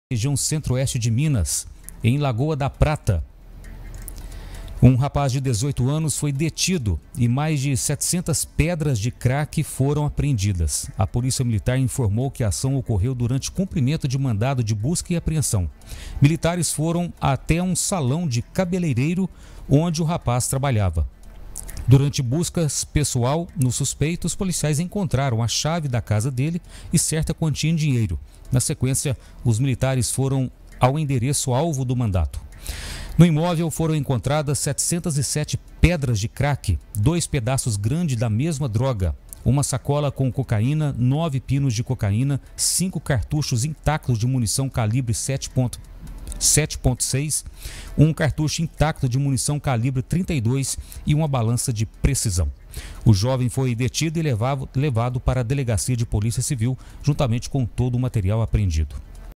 MAIS DE 700 PEDRAS DE CRACK SÃO APREENDIDAS EM LAGOA DA PRATA
I REGIÃO I

Em Lagoa da Prata, um rapaz de 18 anos foi detido e mais de 700 pedras de crack foram apreendidas. Saiba mais informações: